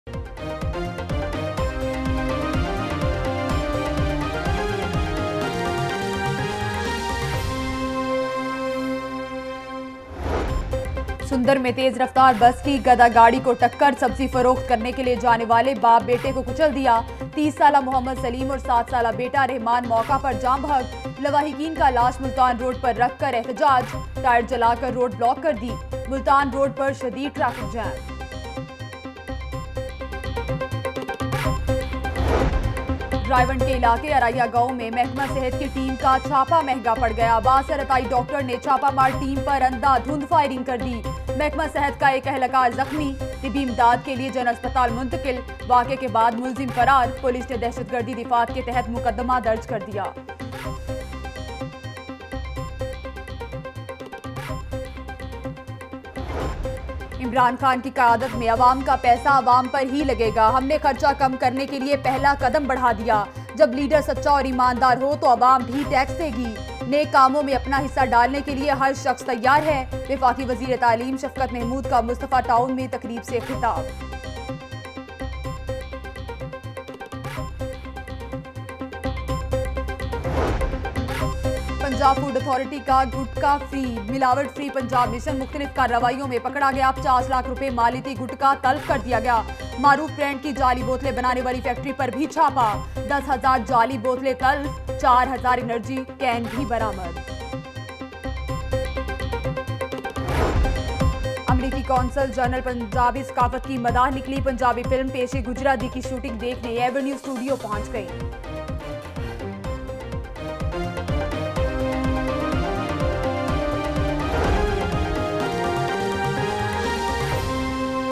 10 AM Headlines Lahore News HD - 09 September 2018
Watch 10 am headlines: An over speeding wagon in Sundar area ran over two people, killing both on the spot -  Public's money will be solely spent on Public under Imran khan's leadership, says Shafqat Mehmood - Punjab Food Authority in action - American Counsel General becomes a fan of Pakistani culture and much more.